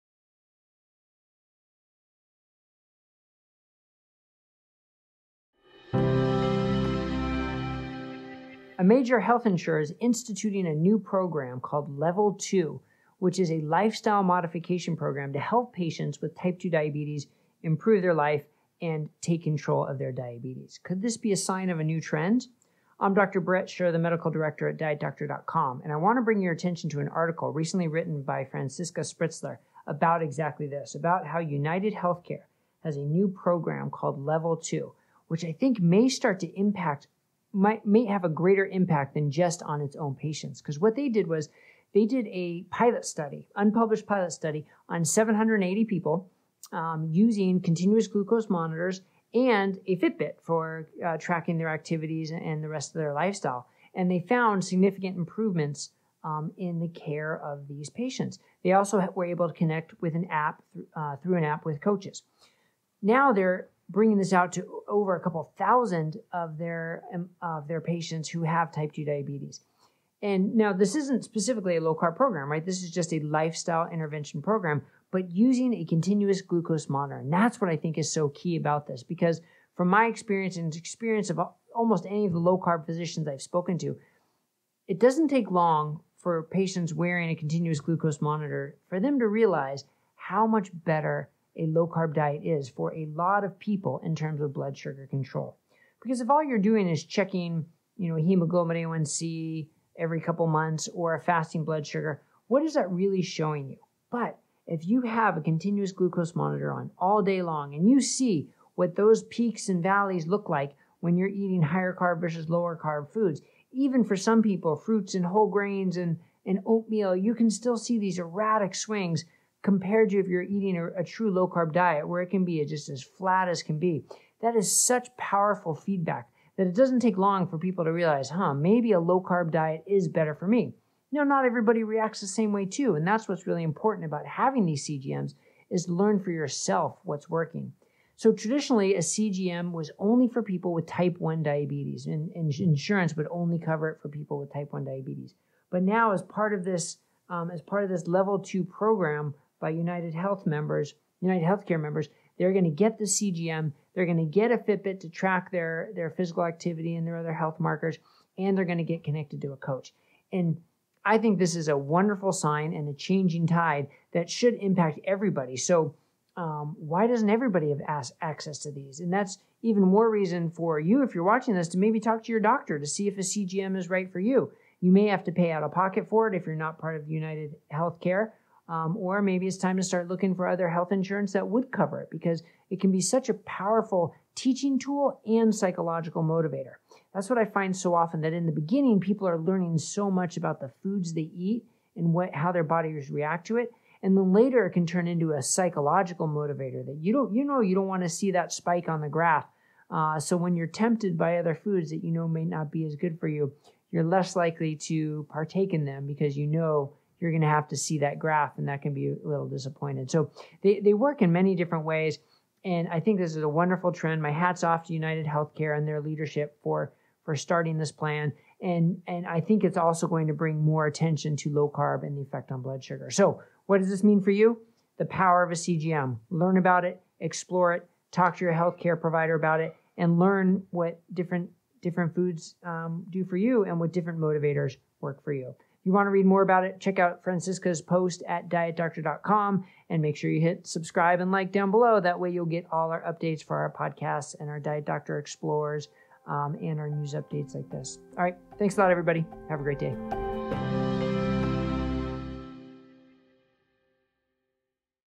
Top health insurer to launch a program for controlling T2D
A major health insurer institutes a new program using CGM and lifestyle interventions to help patients with type 2 diabetes. This could be a great sign of things to come!